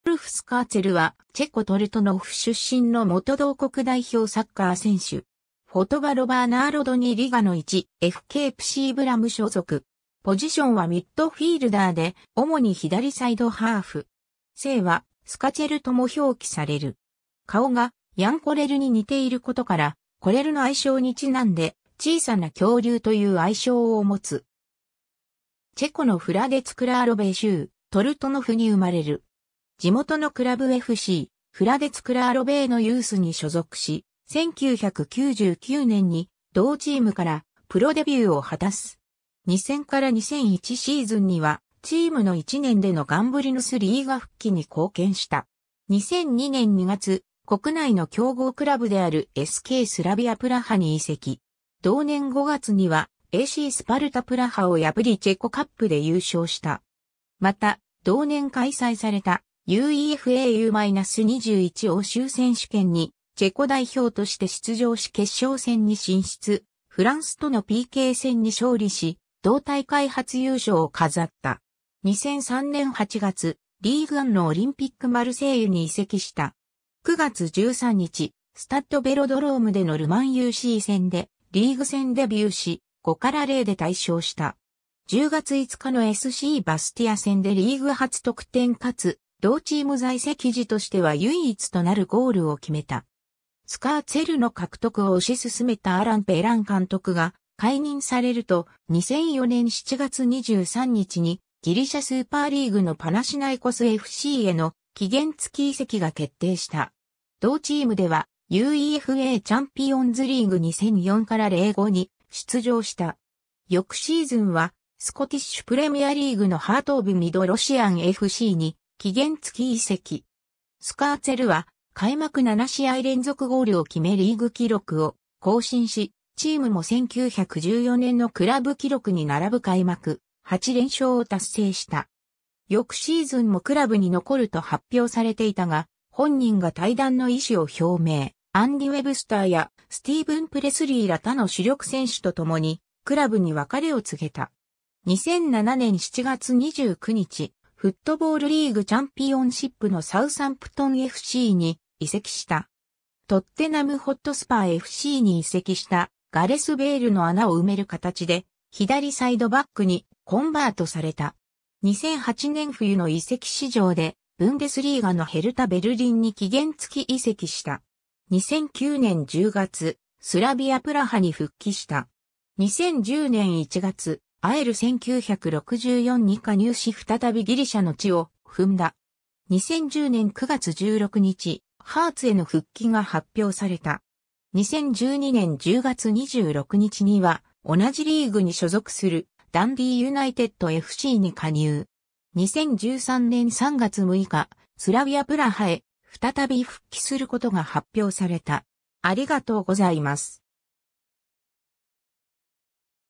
ルドルフ・スカーツェル
FCフラデツ・クラーロヴェーの選手
SKスラヴィア・プラハの選手
オリンピック・マルセイユの選手
ハート・オブ・ミドロシアンFCの選手
ヘルタ・ベルリンの選手
ルドルフ・スカーツェル（Rudolf Skácel, 1979年7月17日 - ）は、チェコ・トルトノフ出身の元同国代表サッカー選手。
フォトバロヴァー・ナーロドニー・リガの1. FKプシーブラム所属。
ポジションはミッドフィールダーで、主に左サイドハーフ。
姓はスカチェルとも表記される。
 顔がヤン・コレルに似ていることから、コレルの愛称にちなんで「小さな恐竜」という愛称を持つ。
 チェコ（当時チェコスロバキア）のフラデツ・クラーロヴェー州トルトノフに生まれる。
地元のクラブFCフラデツ・クラーロヴェーのユースに所属し、1999年に同チームからプロデビューを果たす。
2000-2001シーズンにはチームの1年でのガンブリヌス・リーガ復帰に貢献した。
2002年2月、国内の強豪クラブであるSKスラヴィア・プラハに移籍。
同年5月にはACスパルタ・プラハを破りチェコカップで優勝した。
また、同年開催されたUEFA U-21欧州選手権にチェコ代表として出場し決勝戦に進出、フランスとのPK戦に勝利し同大会初優勝を飾った。
 2003年8月、リーグ・アンのオリンピック・マルセイユに移籍した。
9月13日、スタッド・ヴェロドロームでのル・マンUC戦でリーグ戦デビューし、5-0で大勝した。
10月5日のSCバスティア戦でリーグ初得点かつ同チーム在籍時としては唯一となるゴールを決めた。
スカーツェルの獲得を推し進めたアラン・ペラン監督が解任されると、2004年7月23日にギリシャ・スーパーリーグのパナシナイコスFCへの期限付き移籍が決定した。
同チームではUEFAチャンピオンズリーグ 2004-05に出場した。
 翌シーズンはスコティッシュ・プレミアリーグのハート・オブ・ミドロシアンFCに期限付き移籍。
スカーツェルは開幕7試合連続ゴールを決めリーグ記録を更新し、チームも1914年のクラブ記録に並ぶ開幕8連勝を達成した。
翌シーズンもクラブに残ると発表されていたが、本人が退団の意志を表明、アンディ・ウェブスターやスティーヴン・プレスリーら他の主力選手と共にクラブに別れを告げた。
 2007年7月29日、フットボールリーグ・チャンピオンシップのサウサンプトンFCに移籍した。
トッテナム・ホットスパーFCに移籍したガレス・ベイルの穴を埋める形で左サイドバックにコンバートされた。
2008年冬の移籍市場でブンデスリーガのヘルタ・ベルリンに期限付き移籍した。
 2009年10月、スラヴィア・プラハに復帰した。
 2010年1月、AEL 1964に加入し再びギリシャの地を踏んだ。
 2010年9月16日、ハーツへの復帰が発表された。
2012年10月26日には同じリーグに所属するダンディー・ユナイテッドFCに加入。
2013年3月6日、スラヴィア・プラハへ再び復帰することが発表された。